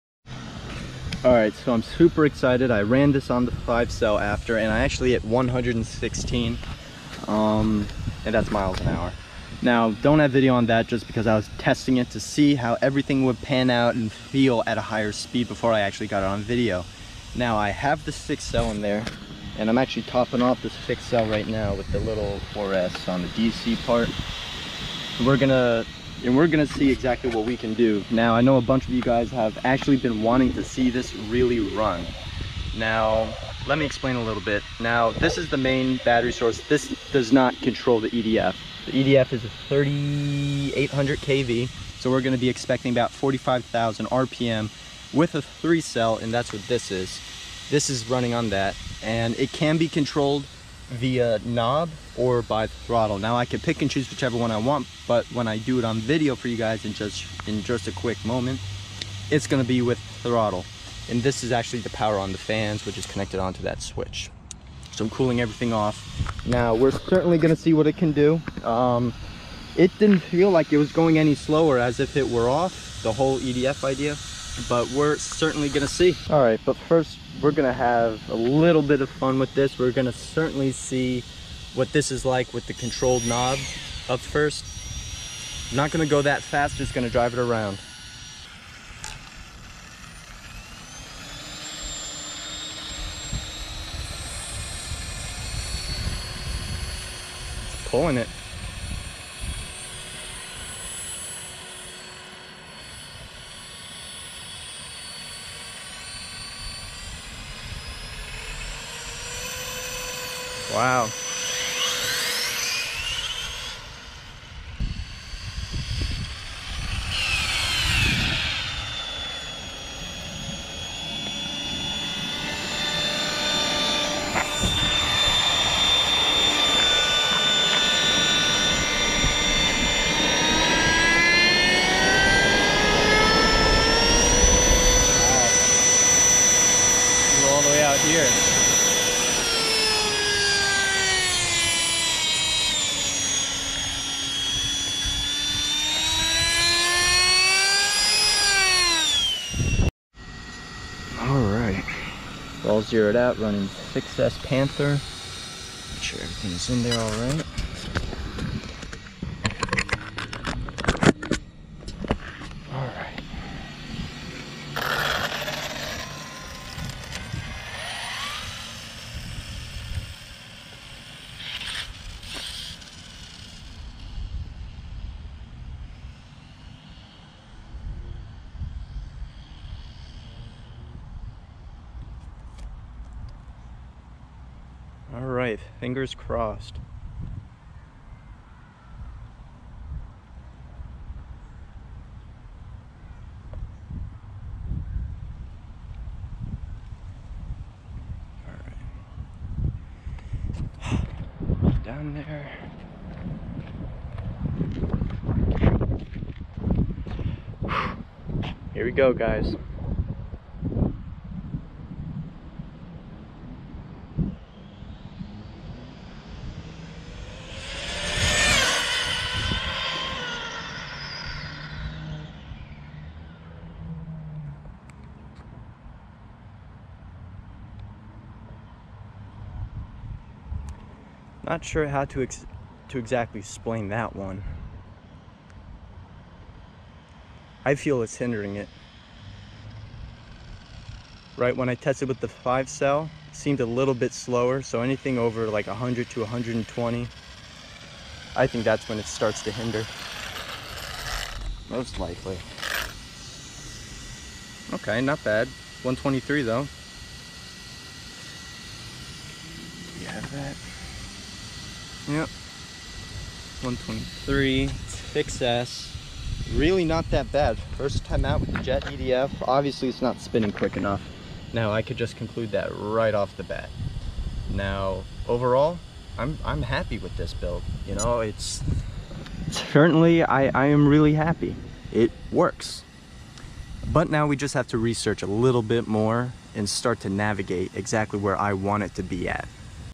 Jet Typhon 200+ KMH/ Hybrid Powered RC Car EDF Test
The purpose of this video is to test and eventually build up to a future project.